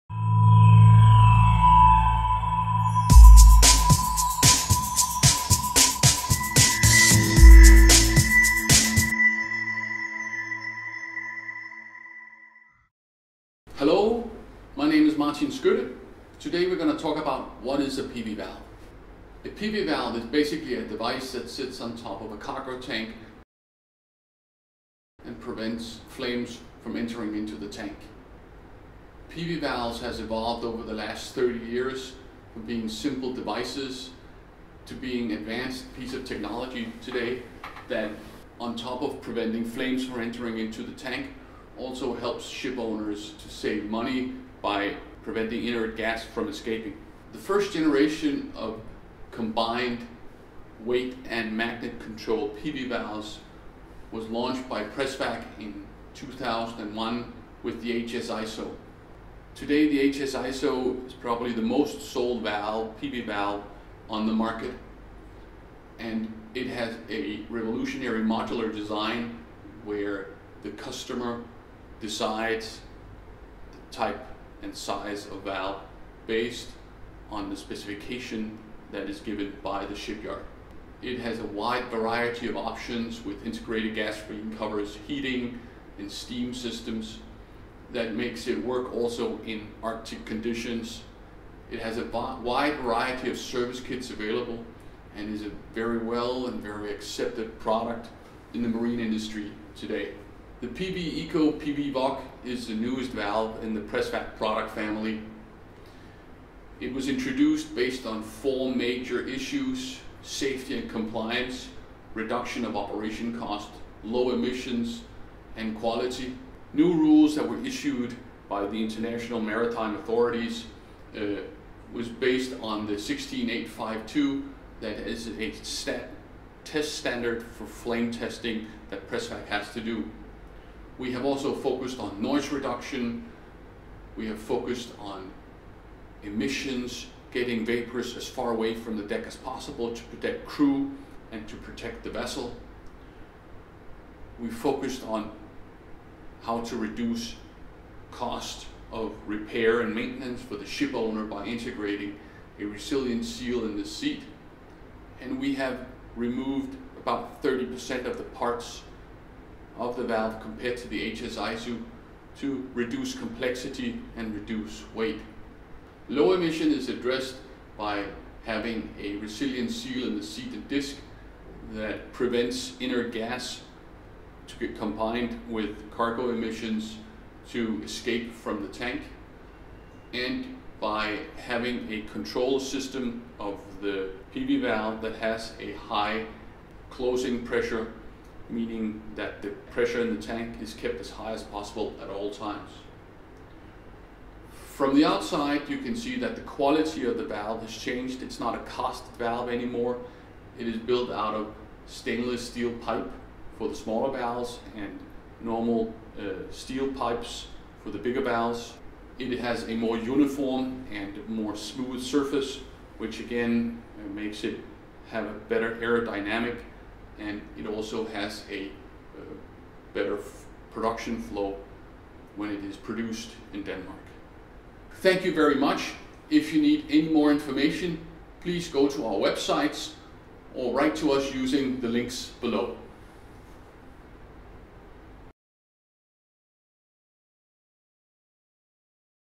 What is a PV Valve?
Introduction of Pressure Vacuum (PV) Valves used onboard marine and offshore tankers by Martin Skytte from Pres-Vac.